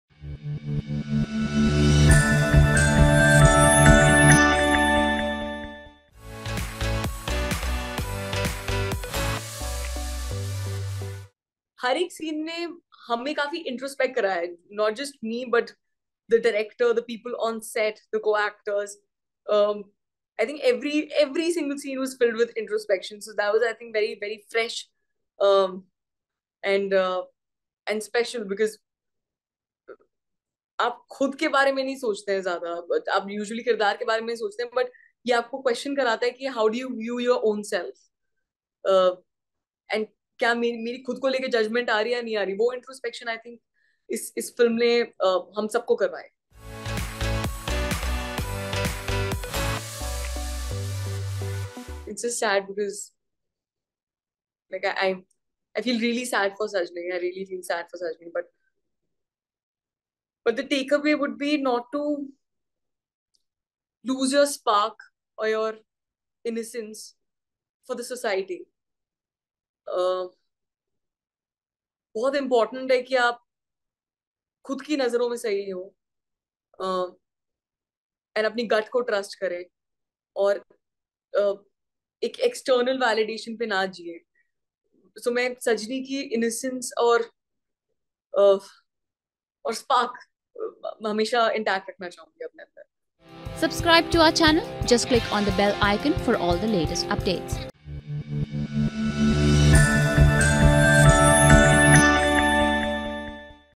Exclusive Interview with Bollywood Actress Radhika Madan
Actress Radhika Madan, who essayed the titular character in the mystery film 'Sajini Shinde Ka Viral Video' opened up on how the movie made them introspect themselves a lot and wants to keep the 'spark and innocence' of her role intact in herself. Talking to IANS, Radhika shared one takeaway from her character Sajini.